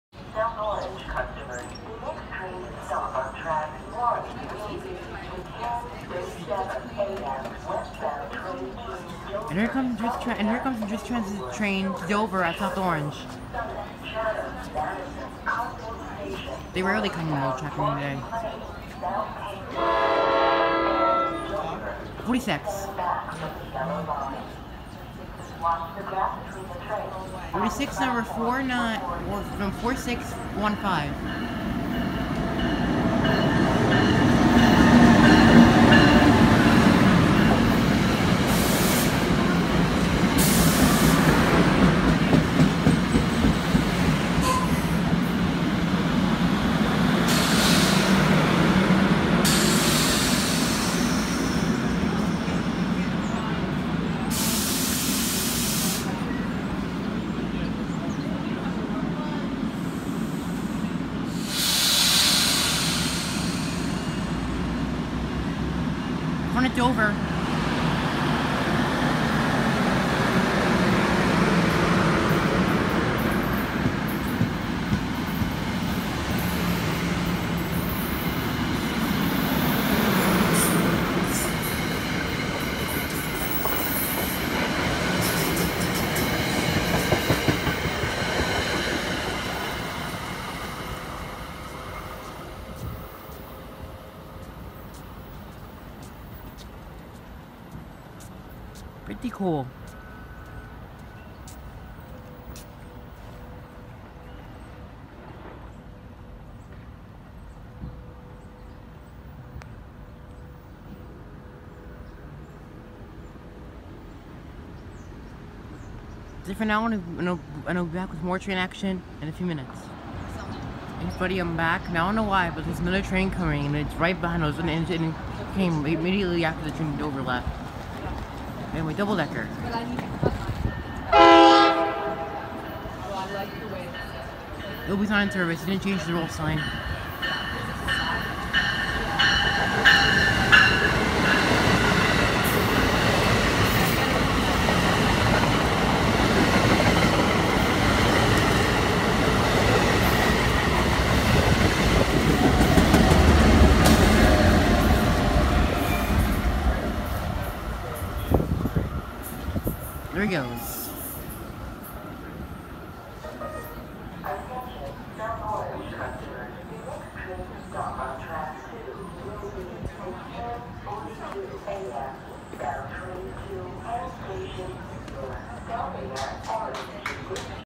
2 NJT trains at South Orange, NJ 10/20/17
Me and my dad decided to head into NYC today! Before our train arrived, I filmed 2 outbound trains towards Dover.

1: NJT ALP-46 pulling a comet set to Dover

2: NJT ALP-46A pushing a multilevel set unscheduled